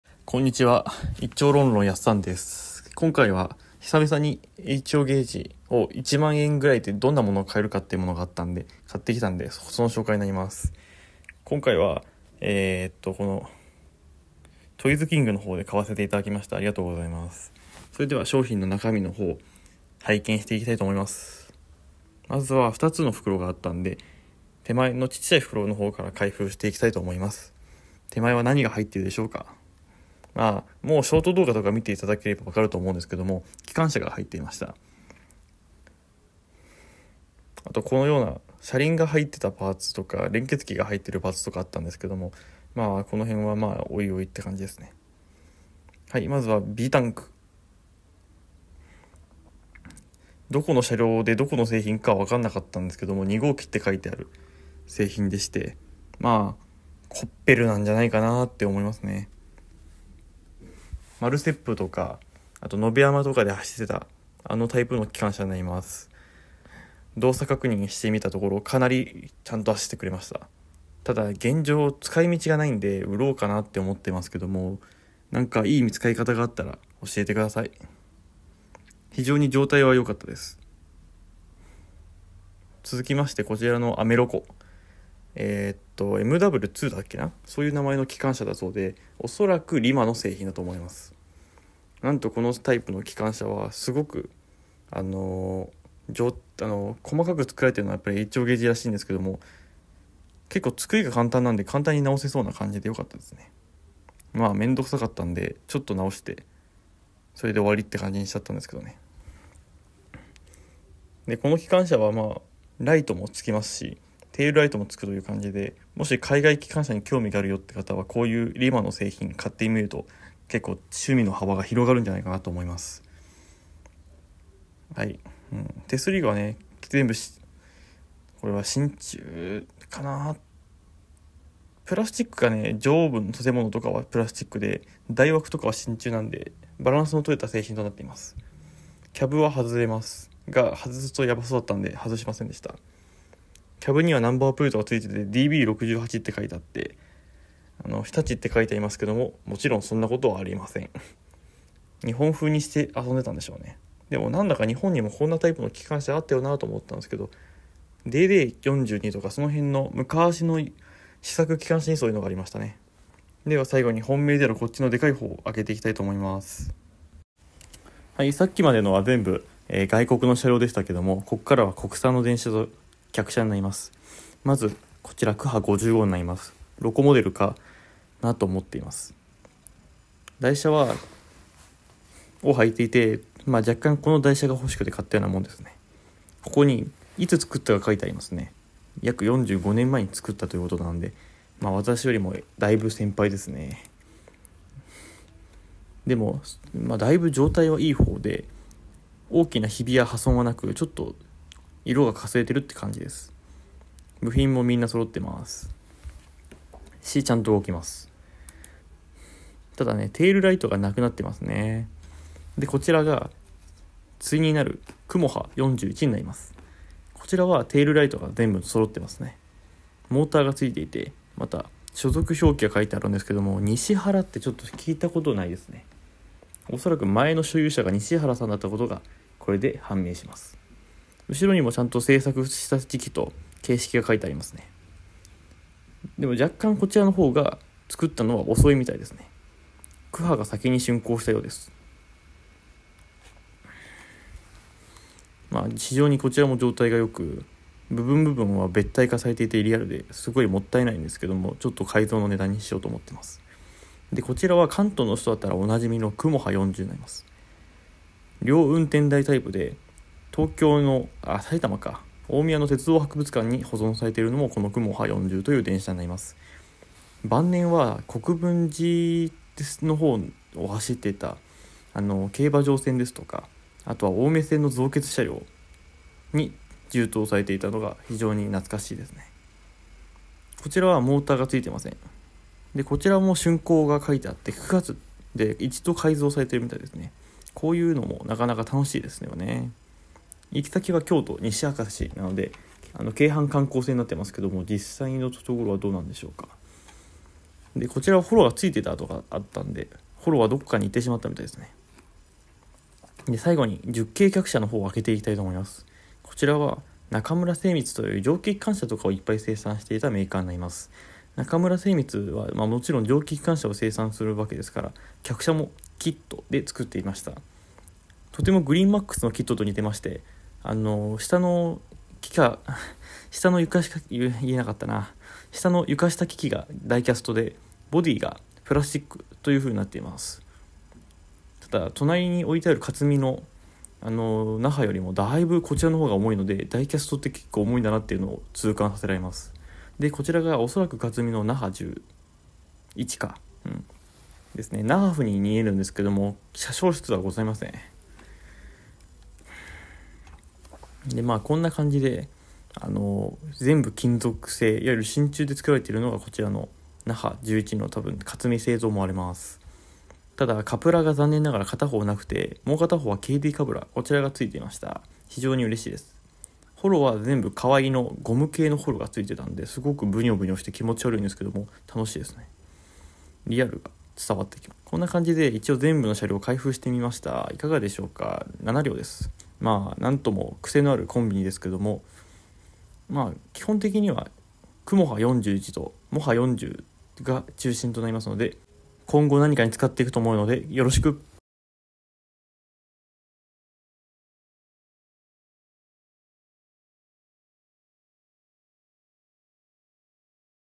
45年前に作られた思い出のhoゲージを開封
「咲いた花びらは後は土へと還るだけ」前略土の上寄りでもお馴染みのこのフレーズですが土に還る前にもう一花咲かしたいものです。さて今回は中古の鉄道模型の開封ですがダメージも少なくいい感じのものですがやはりイマイチな物。姿を変えて楽しんでいきましょう